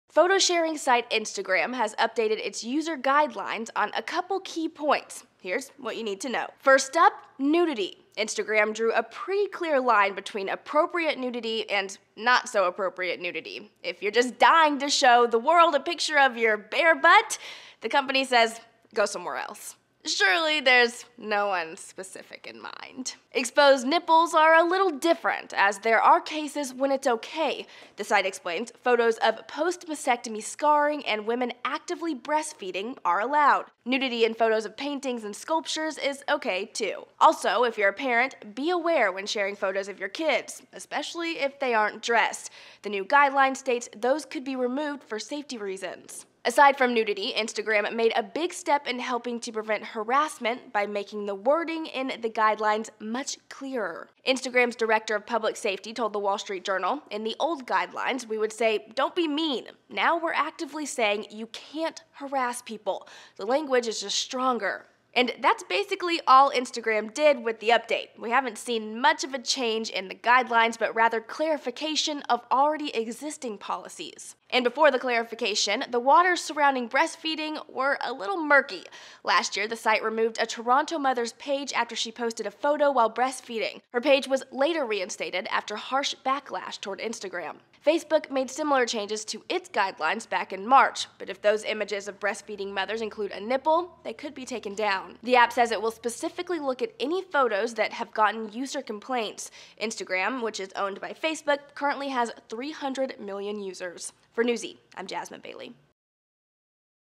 Instagram OKs Breastfeeding Pics In Guideline Update
Instagram has updated its community guidelines, elaborating on nudity and harassment policies.